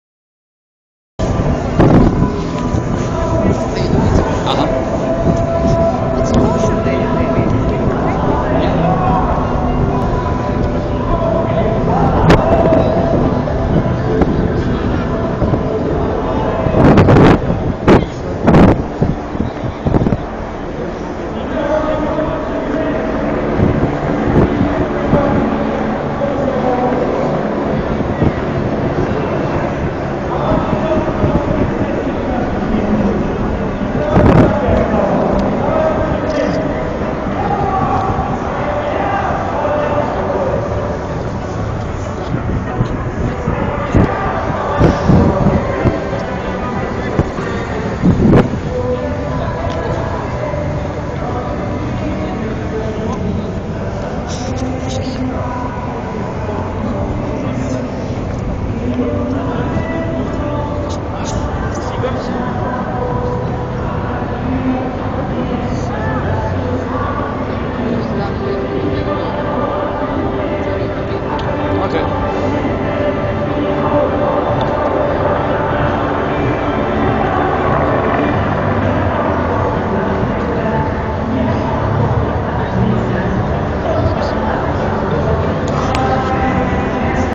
4 March 2012 Walking to Red Square
I had no idea that we were going to be in Moscow for the election. We went to check it out. We walked from Pushkenskeya to Red Square.